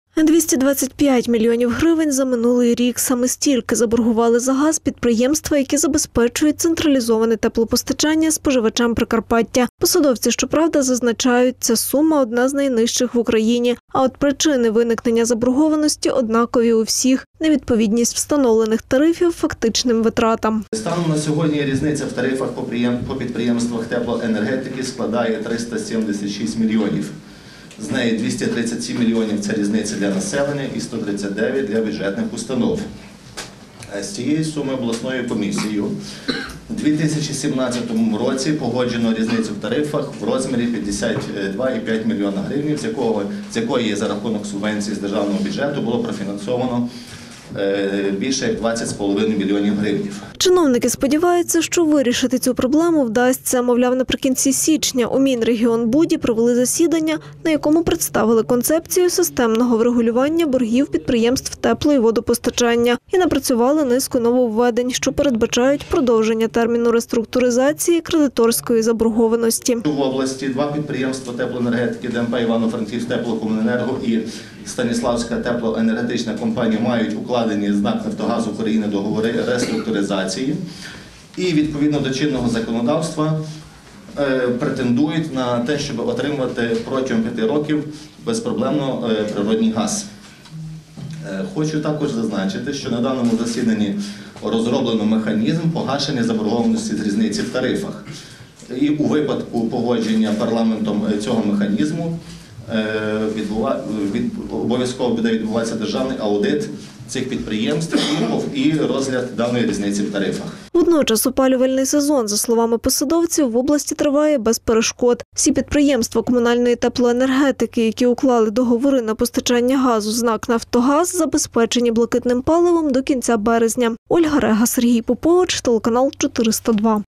За 2019 рік підприємства комунальної теплоенергетики заборгували за газ 225 млн грн
Посадовці, щоправда, зазначають: ця сума - одна з найнижчих в Україні.